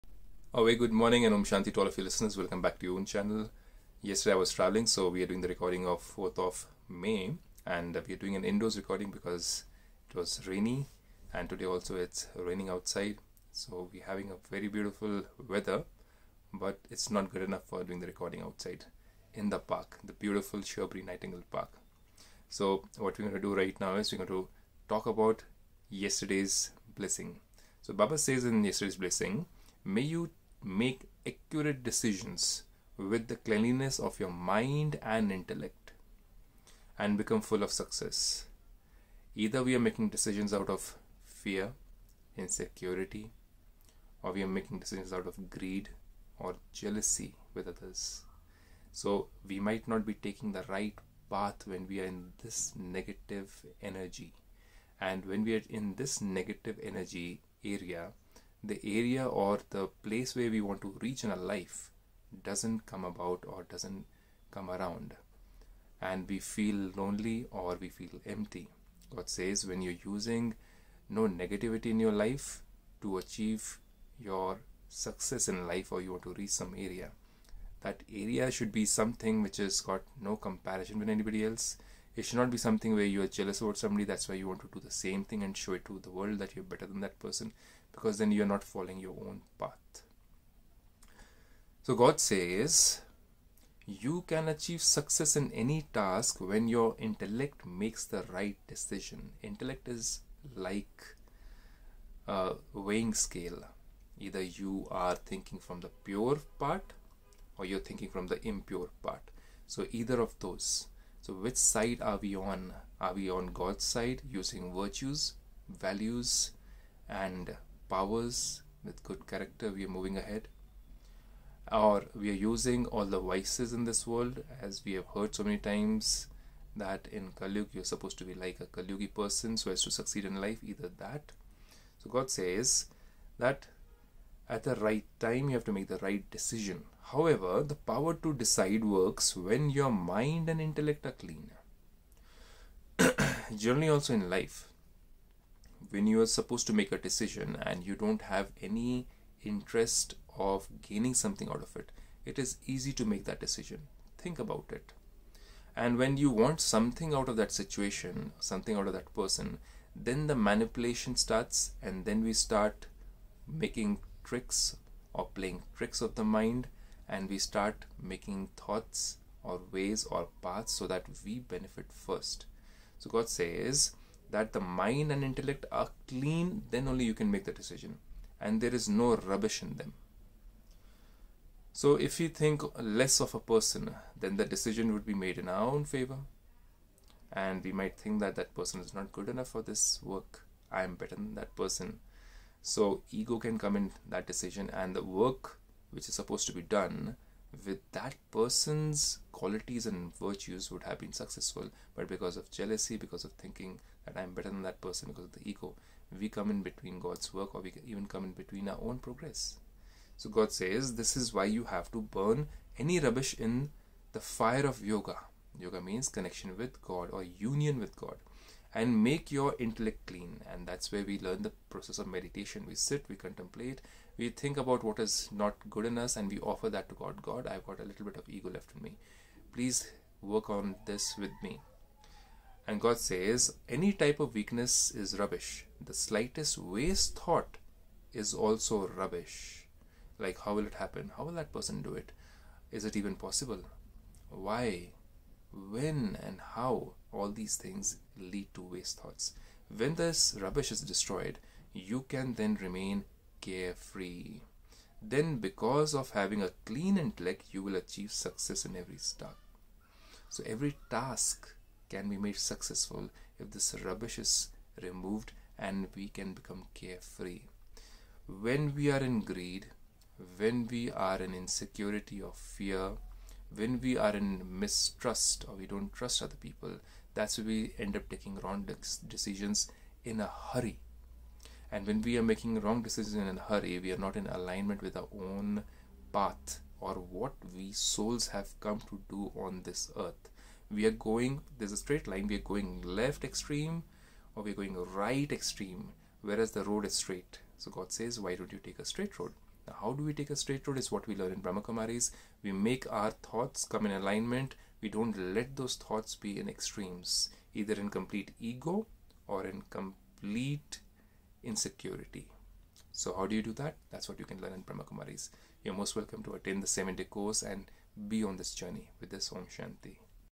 With cleanliness of mind & intellect one can achieve everlasting success , using the power to decide
Day -93 With cleanliness of mind and intellect one can achieve everlasting success by using the power to decide .

Om Shanti .